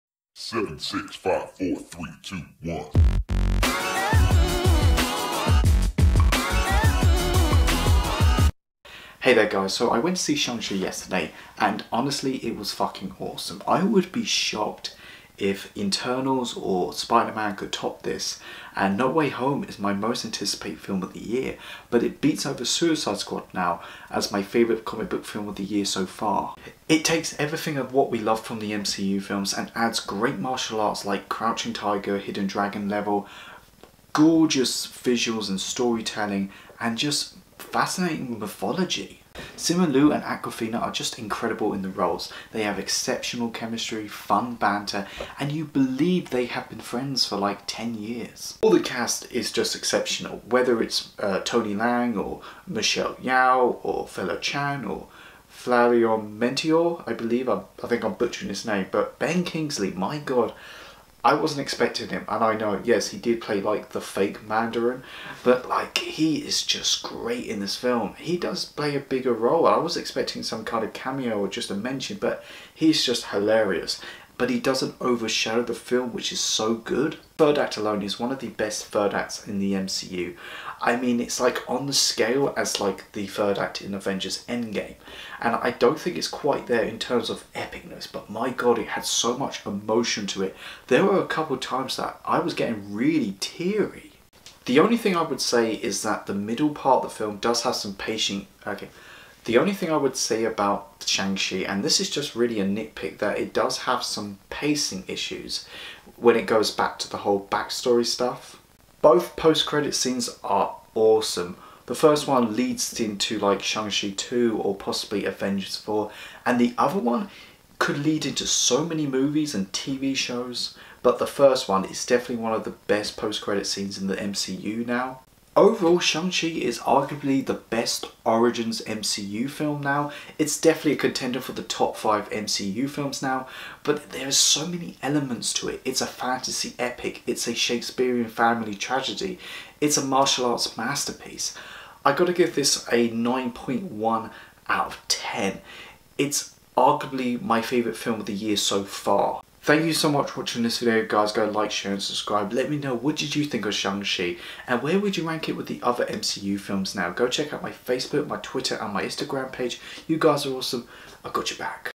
Shang-Chi and the Legend of the Ten Rings - Movie Review
SHANG-CHI AND THE LEGEND OF THE TEN RINGS is a new martial arts adventure in Phase 4 of the MCU. Here's my review!